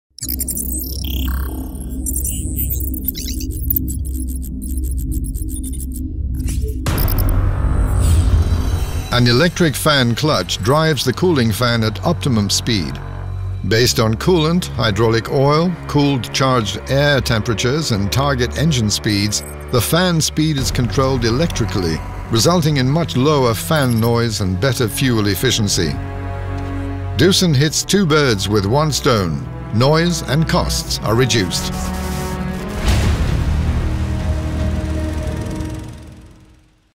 Doosan Wheeled Excavator Electric Fan Clutch | Doosan Equipment Europe
An electric fan clutch drives the cooling fan at optimum speed.  Based on coolant, hydraulic oil, cooled charged air temperatures and target engine speeds, the fan speed is controlled electrically, resulting in much lower fan noise and better fuel efficiency. Doosan hits two birds with one stone: noise and costs are reduced.